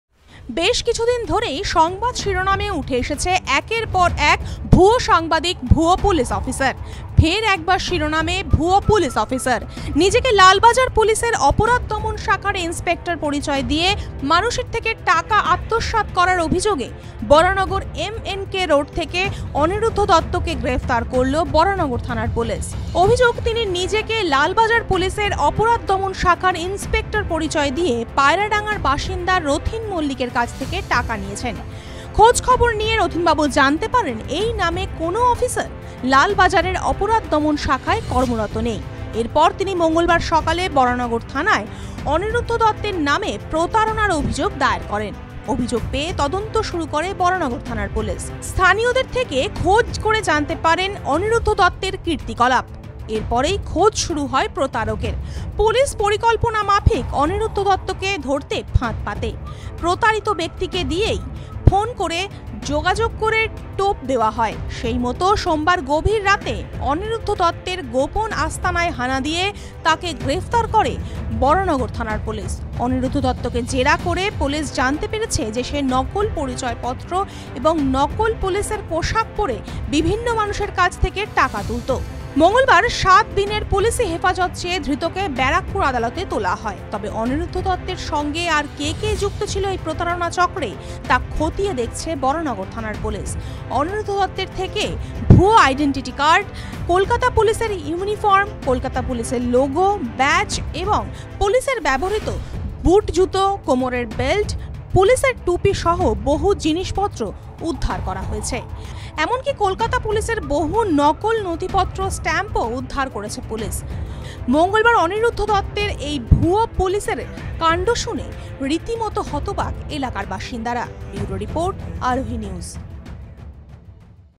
ভুয়ো পুলিশ অফিসার সেজে টাকা আত্মসাৎ-এর অভিযোগ
ফের পুলিশের জালে ভুয়ো পুলিশ অফিসার। নিজেকে লালবাজার পুলিশের অপরাধ দমন শাখার ইন্সপেক্টর পরিচয় দিয়ে মানুষের থেকে টাকা আত্মসাৎ করার অভিযোগ অনিরুদ্ধ দত্তর বিরুদ্ধে। অভিযুক্তকে গ্রেফতার করেছে বরানগর থানার পুলিশ।

@Aarohi News